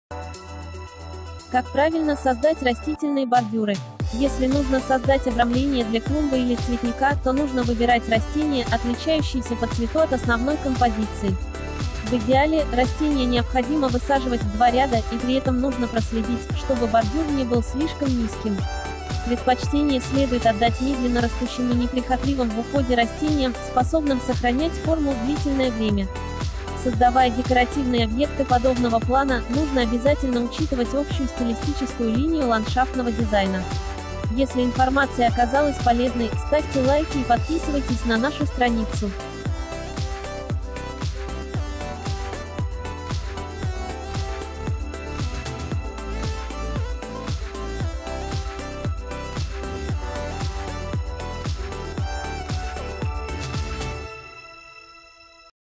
Как правильно создать растительные бордюры
Если нужно создать обрамление для клумбы или цветника, то нужно выбирать растения, отличающиеся по цвету от основной композиции.

В идеале, растения необходимо высаживать в два ряда, и при этом нужно проследить, чтобы бордюр не был слишком низким. Предпочтение следует отдать медленнорастущим и неприхотливым в уходе растениям, способным сохранять форму длительное время.

Создавая декоративные объекты подобного плана, нужно обязательно учитывать общую стилистическую линию ландшафтного дизайна.

Адрес: Москва, 1-я Тверская-Ямская, 21/1